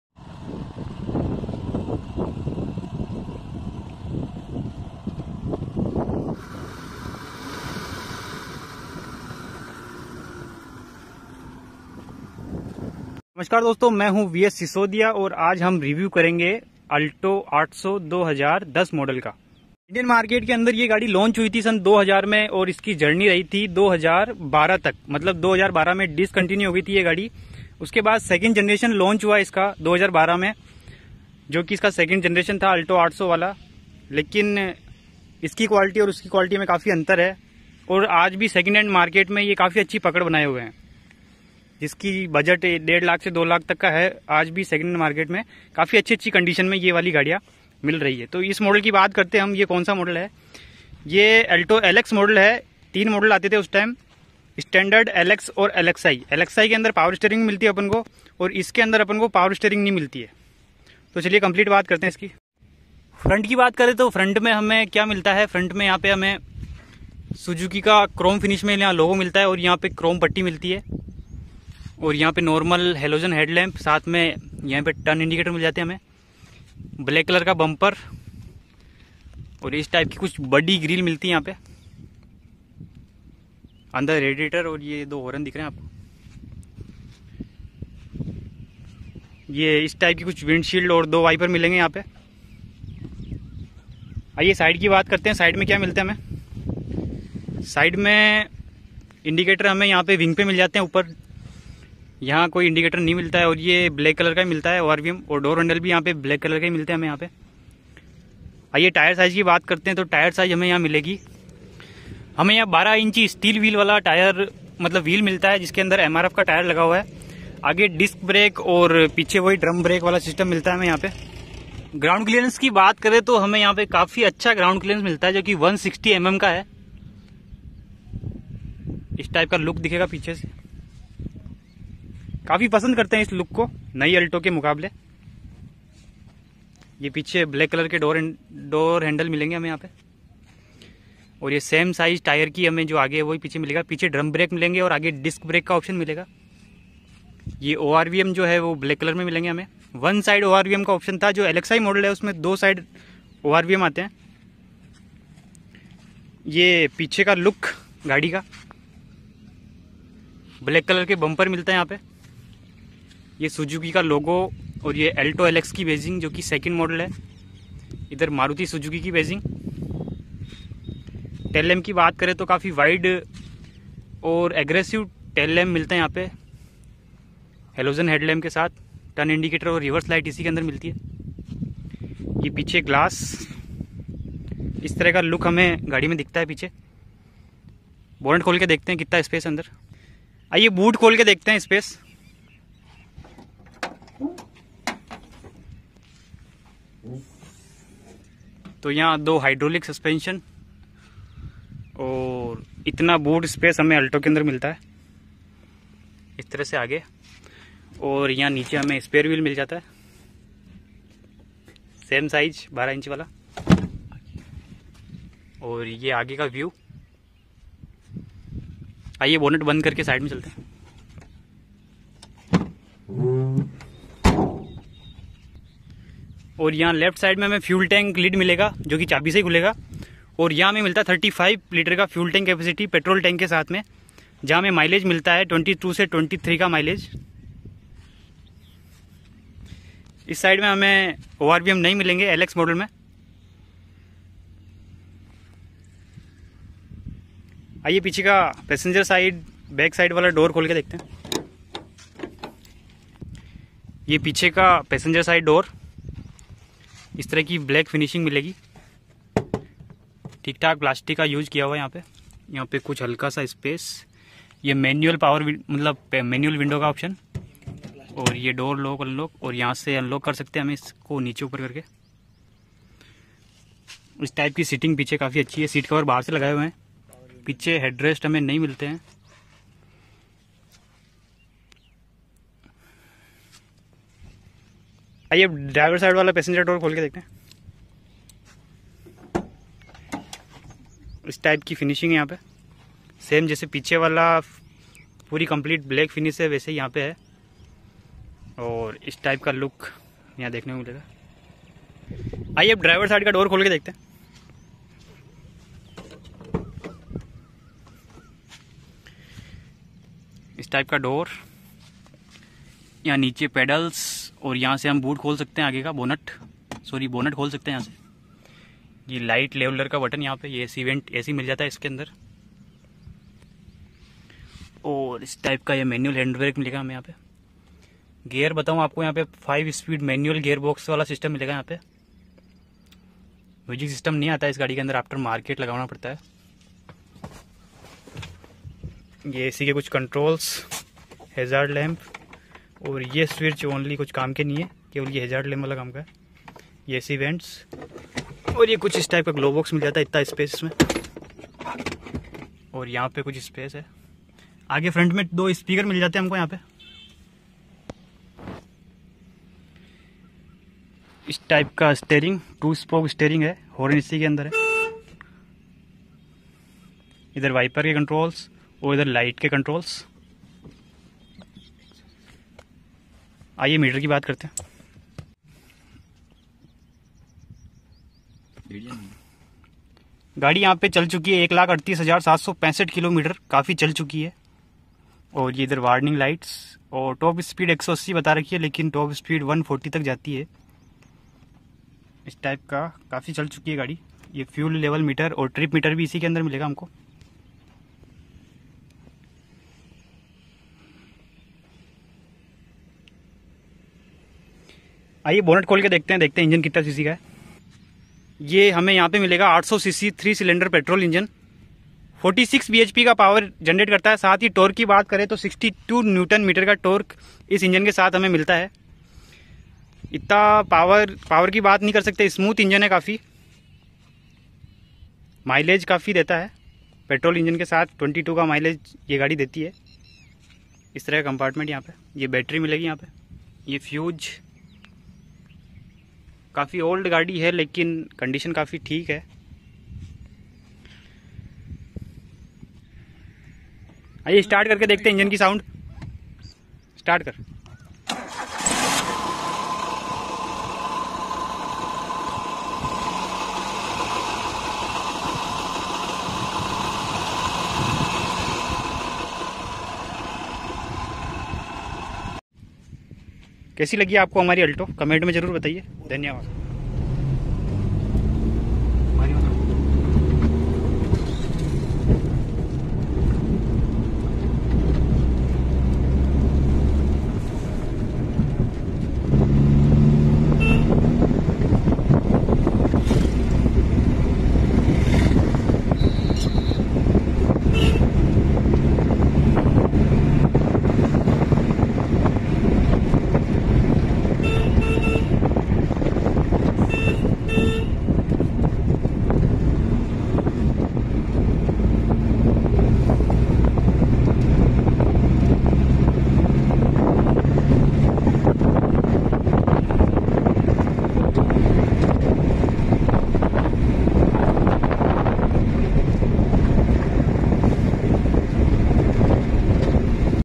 Maruti Suzuki ALTO LX 2010 Model. Full Detail Review After 1.5 लाख Km.
ALTO LX 2010 Model. Full Detail Review.